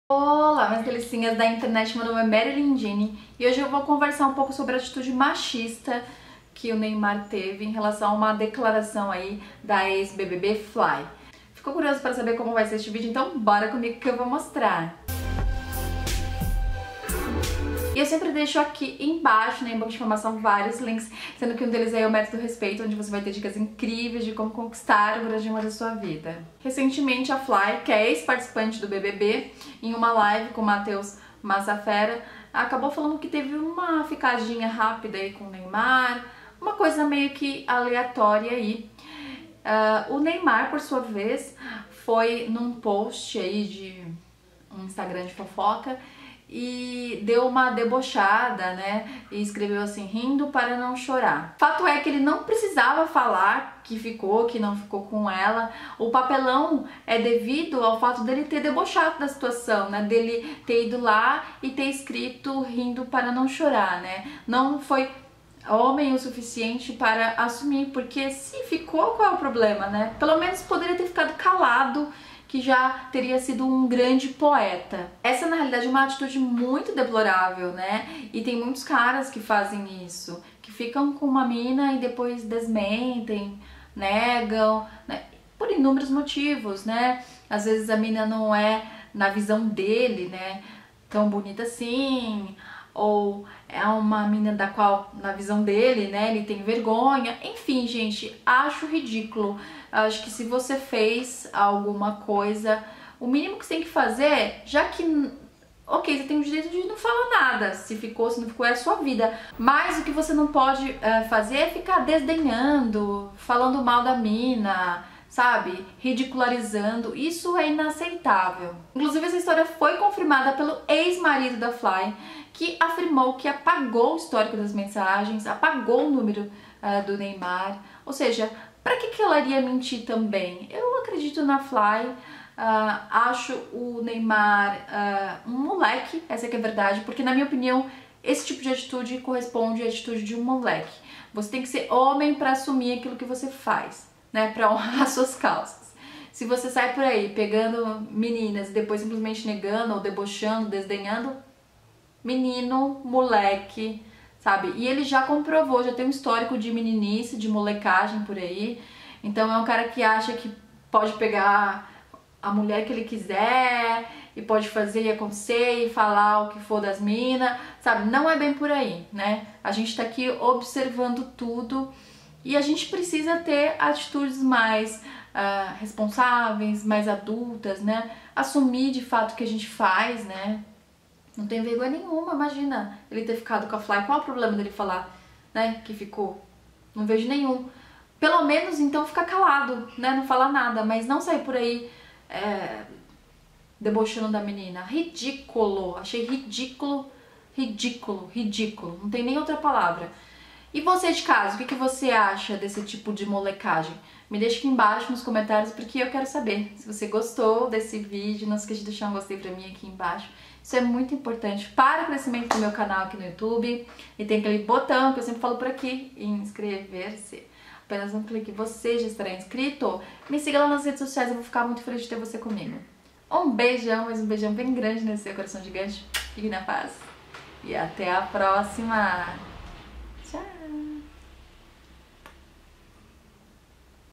NEYMAR DESDENHA DE FLAY DO BBB20
Neste vídeo vou falar da atitude de Neymar que desdenhou de Flay do bbb20

auto estima